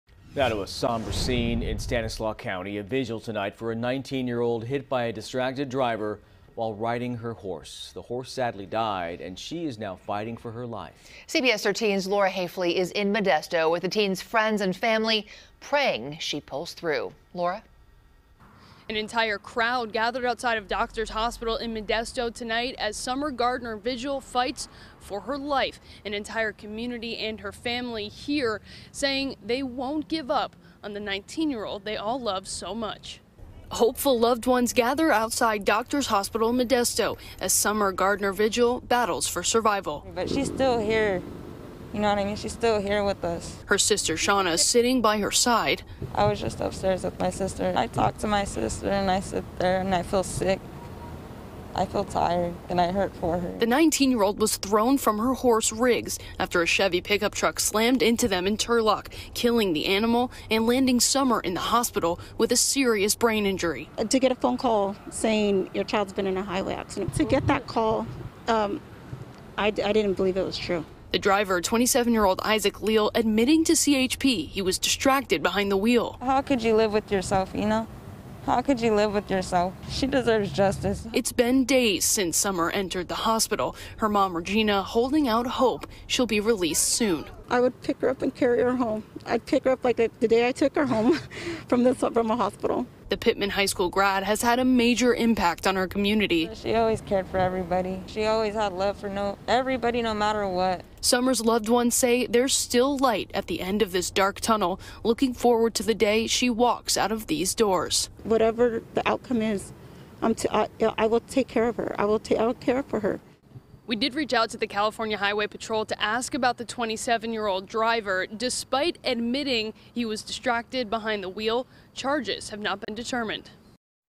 Teen Hit By Car While Riding Horse Fighting For Her Life
A vigil was held Friday night for a 19-year-old who is fighting for her life after being hit by a distracted driver while riding her horse near Turlock.